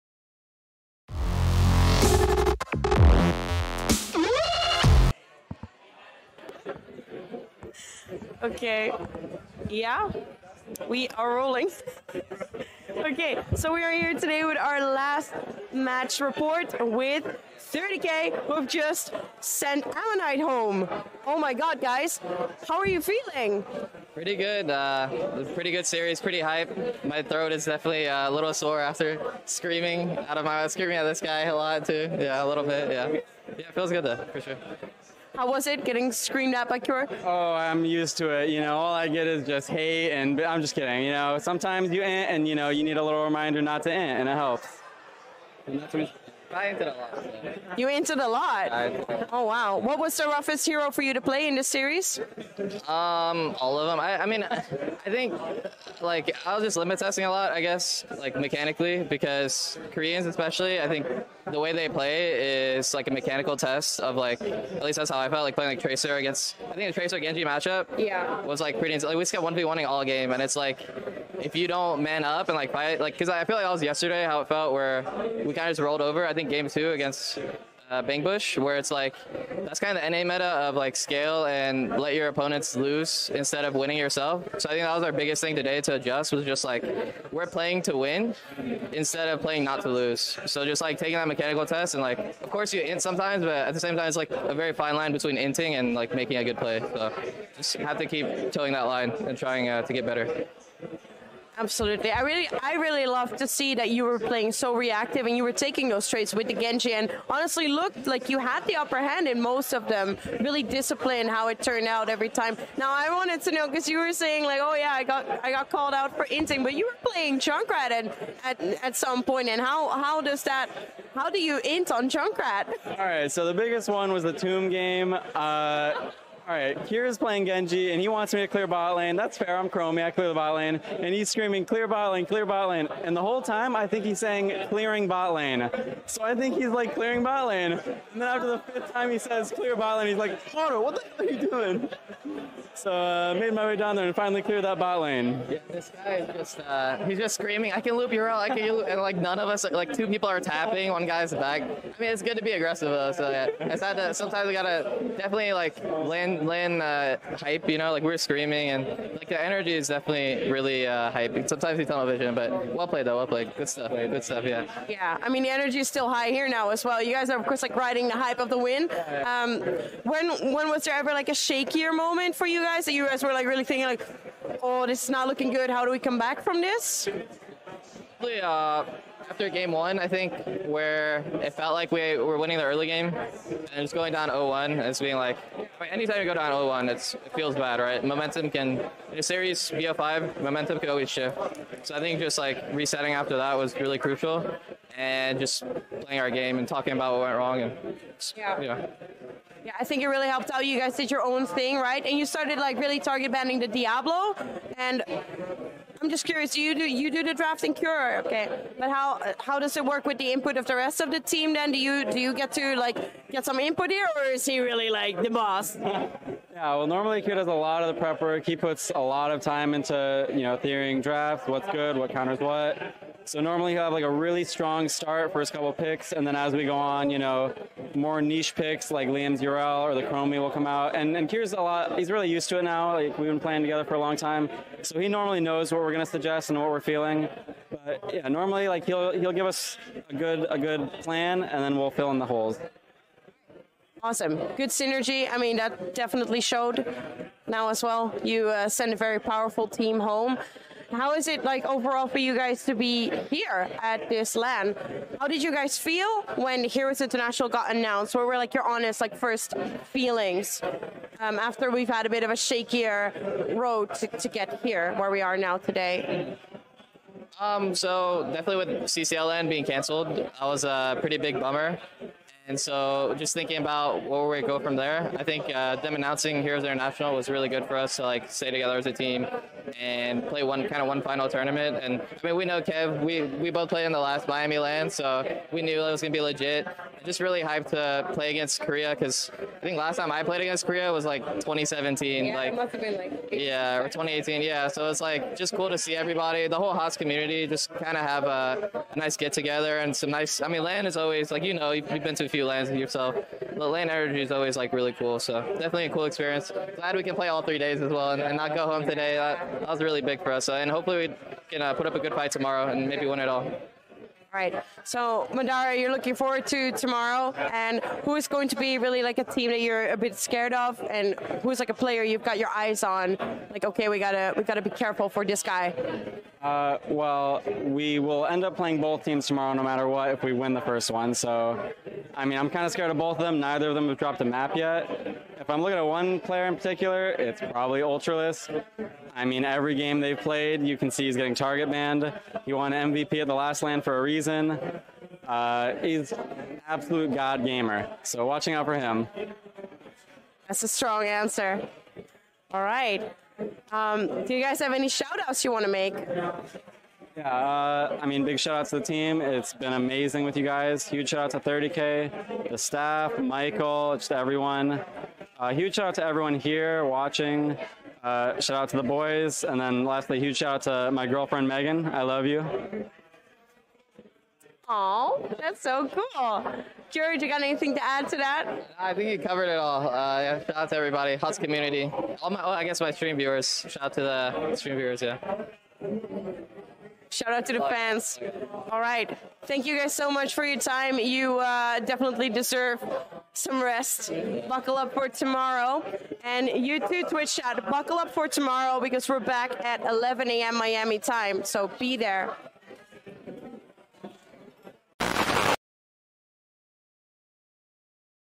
Interview HeroesInternational: 30K Kure & Madara
Interview with 30K's Kure and Madara at the end of the second day of the HeroesInternational. Who can guess why I'm making this face at the beginning of the interview???

The Heroes International is an offline event in Miami with a $11,000 prizepool. The event showcases the best teams out of the three major regions (North America, Europe and Korea) fight to crown themselves the champions of the first Heroes of the Storm International!